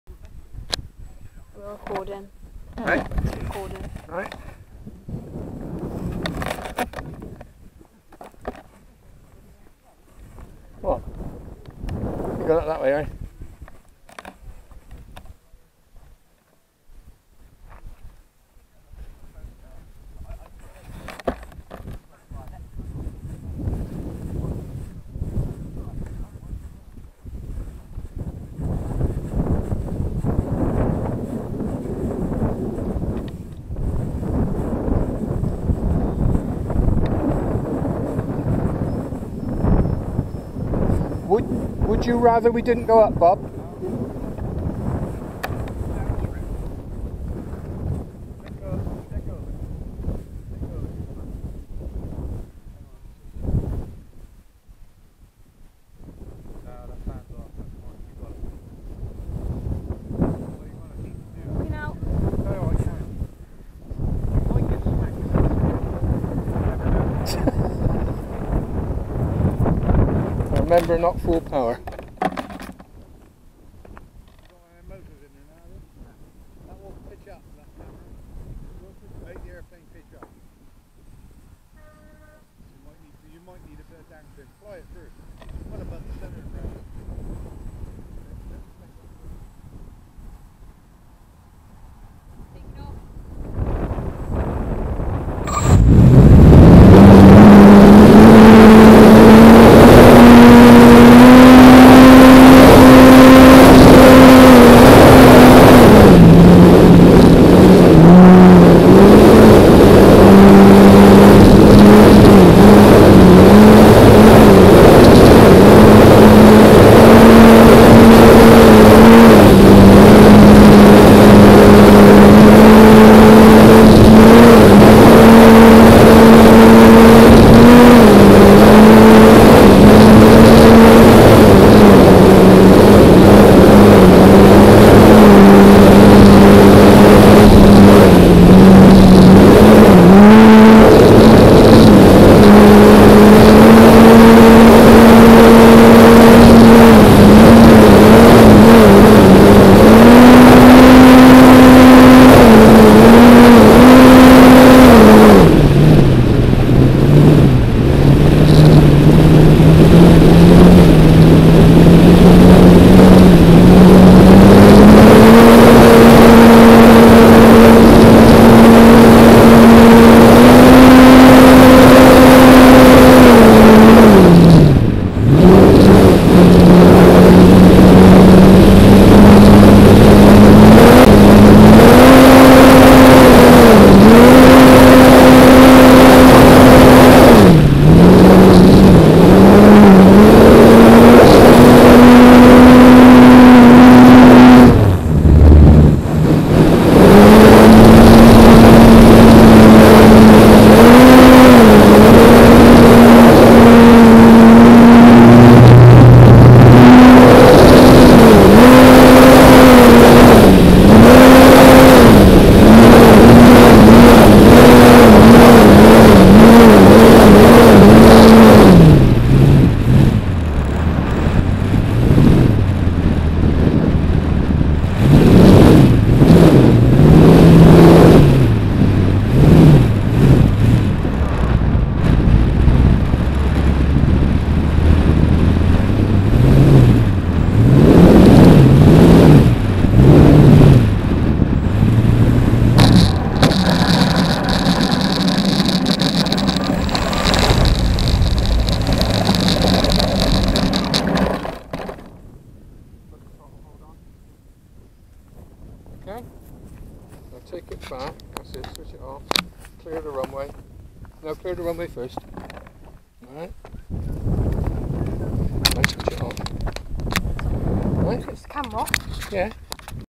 Joes Wot4 Foam e first flight with camera
Joe flies his Wot4 Foam e at Woodspring Wings with the Turnigy full HD cam on top of the wing